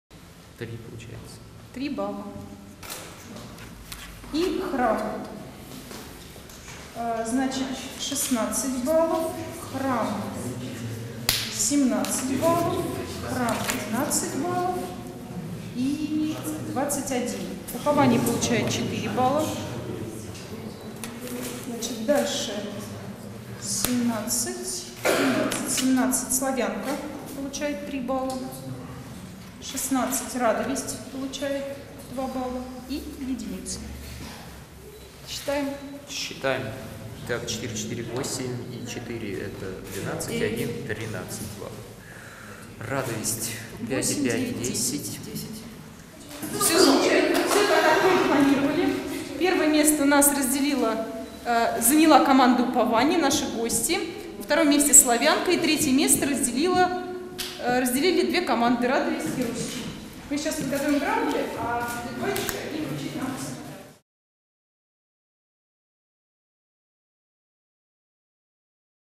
"Святой равноапостальный Князь Владимир. Крещение Руси"
29 марта 2015 года в храмовом комплексе прп. Сергия Радонежского на Рязанке состоялась интеллектуальная молодежная игра, для воспитанников Воскресных школ. В этом году игра была приурочена к празднованию 1000-летия со дня преставления святого равноапостольного великого Князя Владимира.
После прохождения станций все снова собрались в музее, где всех участников ждало подведение итогов .